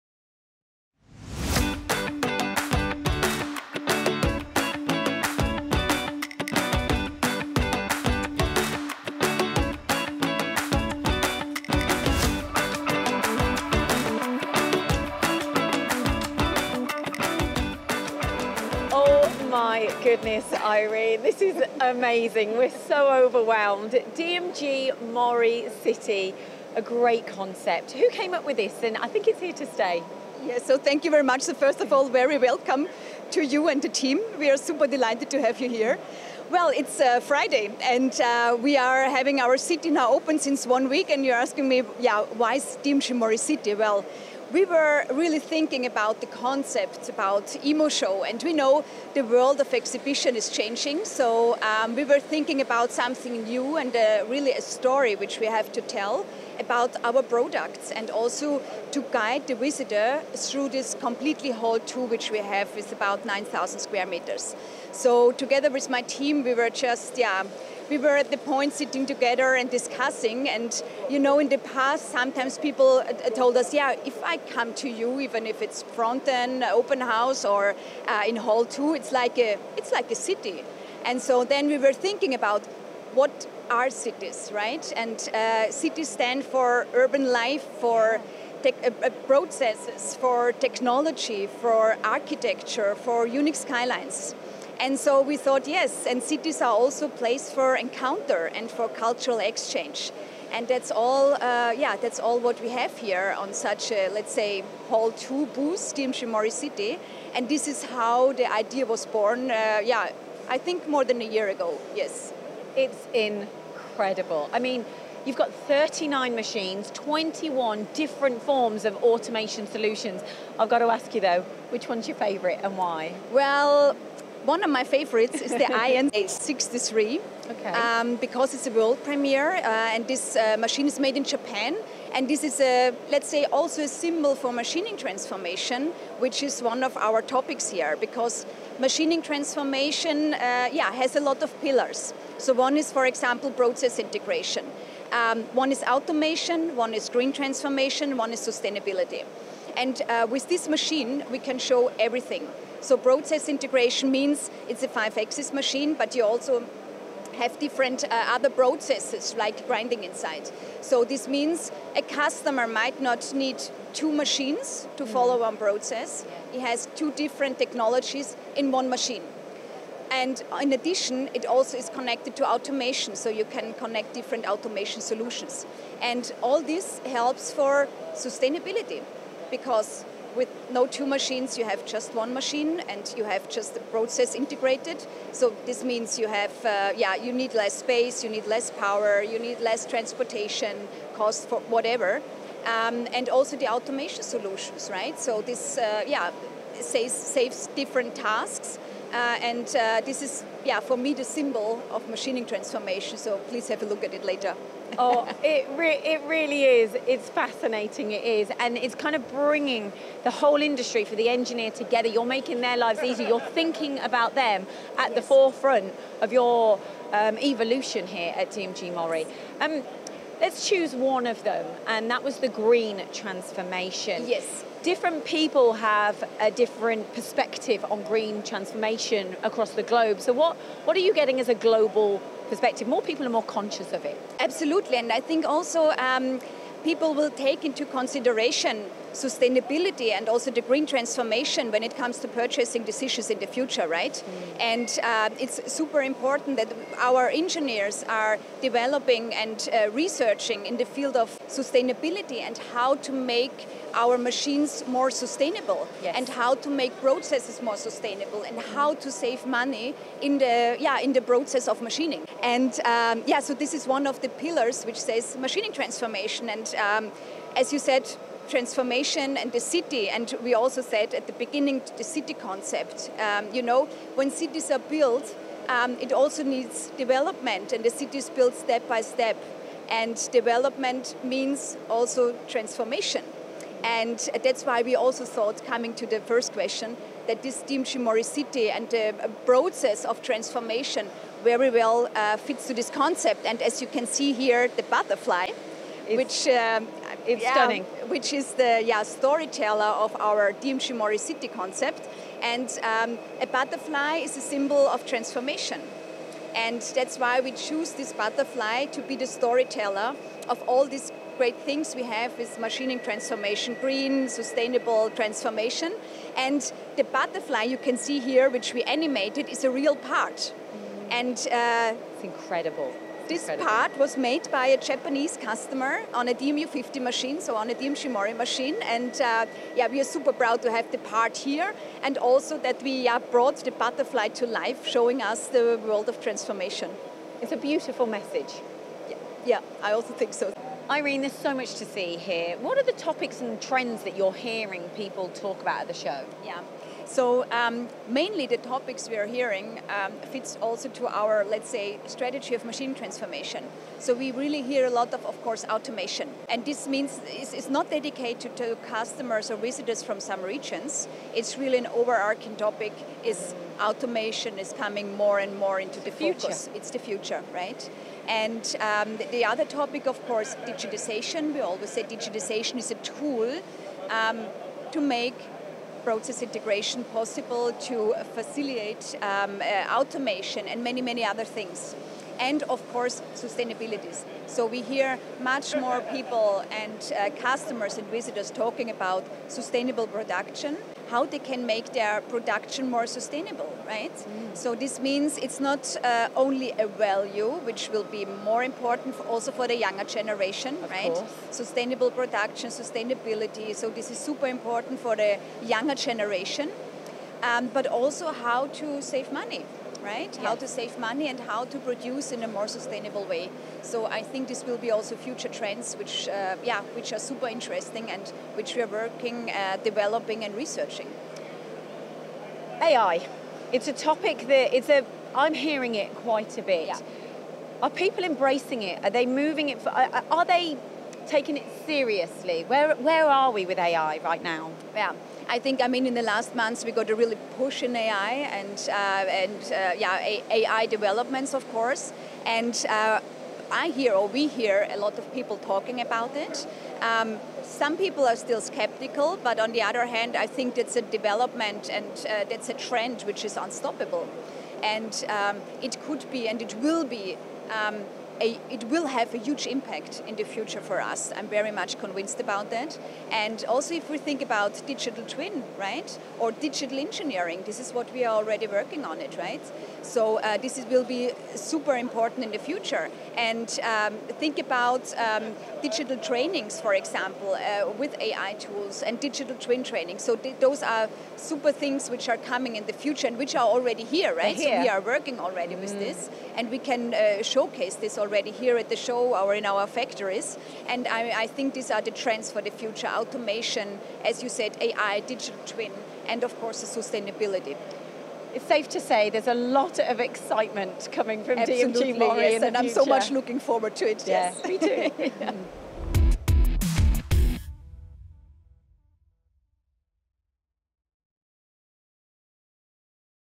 DMG MORI City - Machining Transformation explained
If you didn't have the opportunity to visit the awe-inspiring DMG MORI City during this year's EMO event in Germany, you missed out on something truly exceptional. Lyndsey Vickers had the privilege to engage in a detailed conversation with Irene Bader, delving into the theme behind which the machine tool manufacturer is placing its focus: Machining Transformation (MX). Centred around the four key pillars of MX, namely Process Integration, Automation, Digital Transformation (DX), and Green Transformation (GX), DMG MORI will showcase innovative technologies and solutions that will shape the future of manufacturing.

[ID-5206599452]